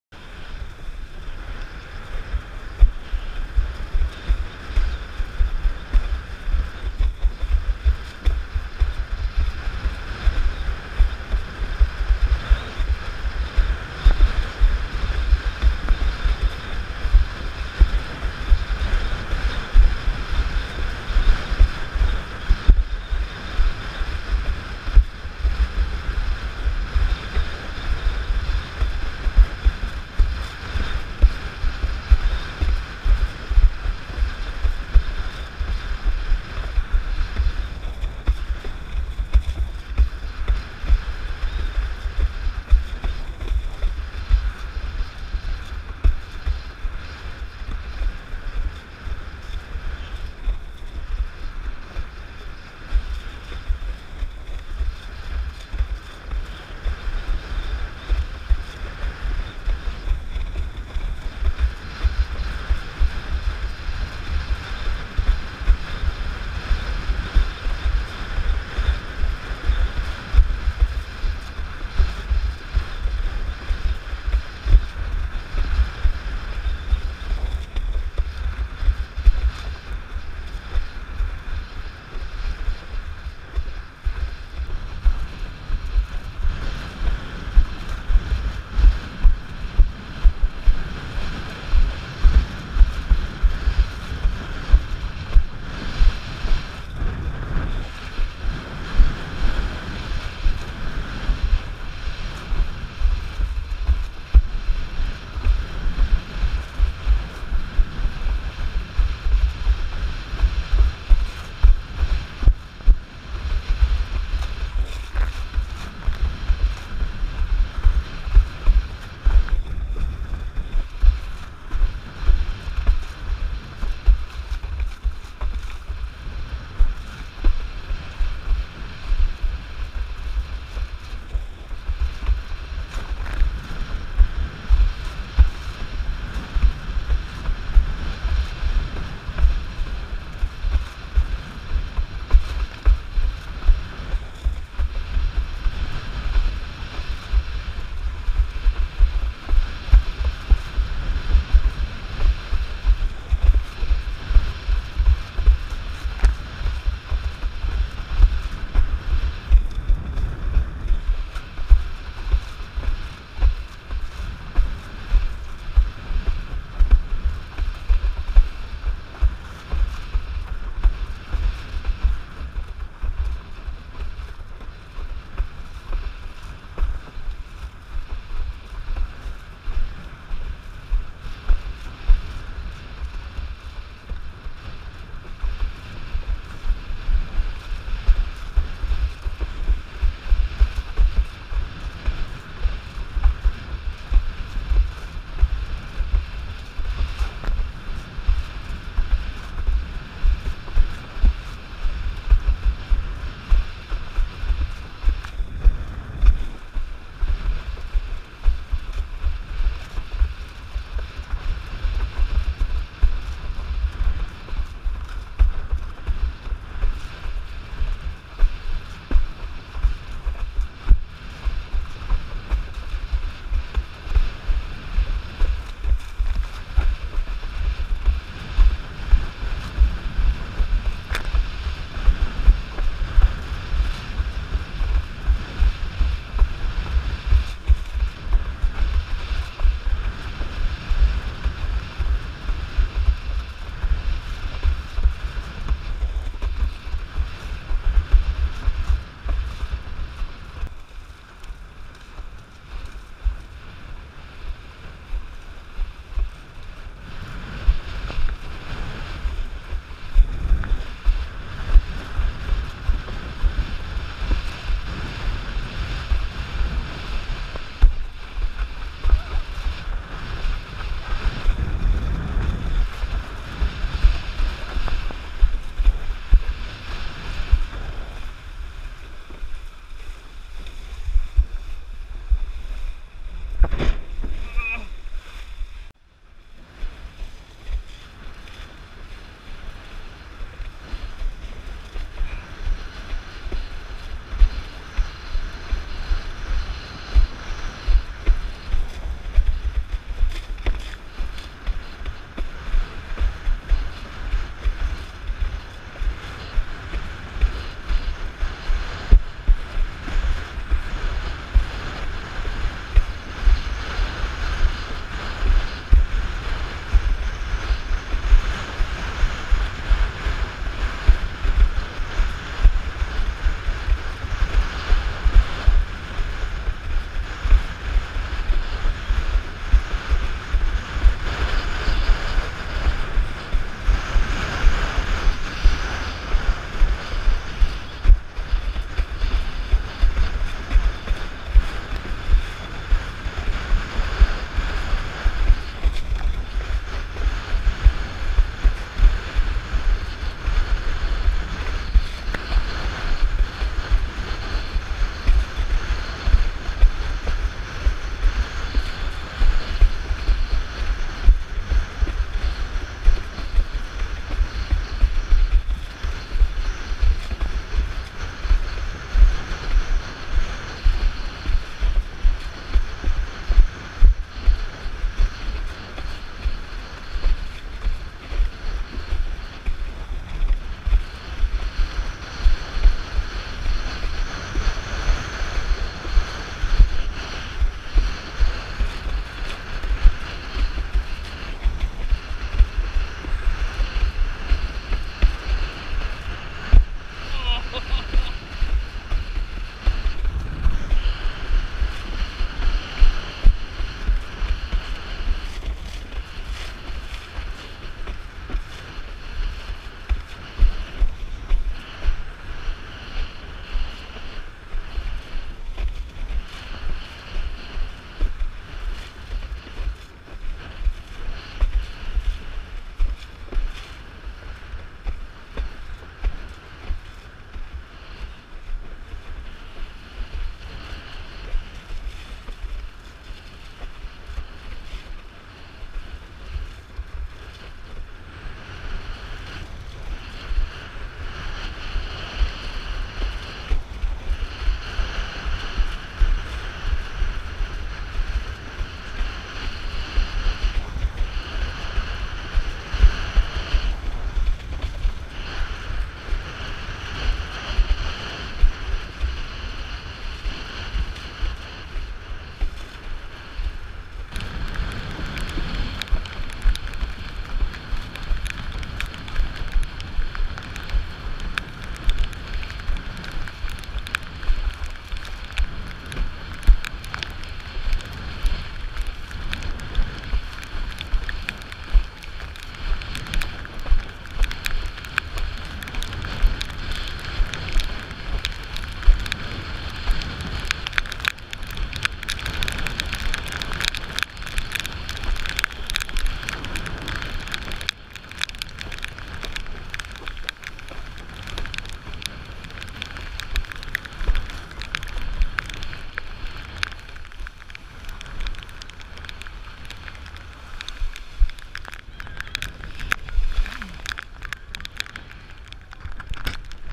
4 lipca 2017 zegrze lusi i 63
Windsurfing na zegrzu.
Starboard 94 2009
Loftsail 6,3 2014
maszt Maverix 75
selekt hotrod 33